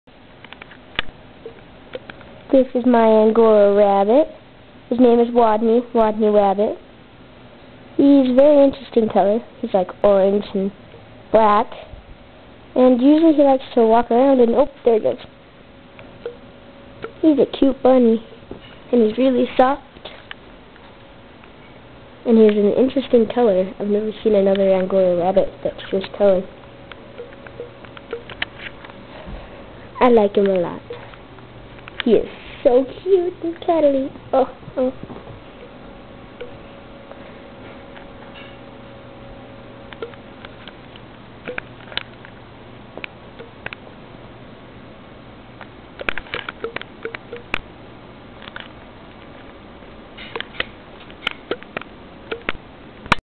my cute angora rabbit
this is my angora rabbit wodney, wodney wabbit hehe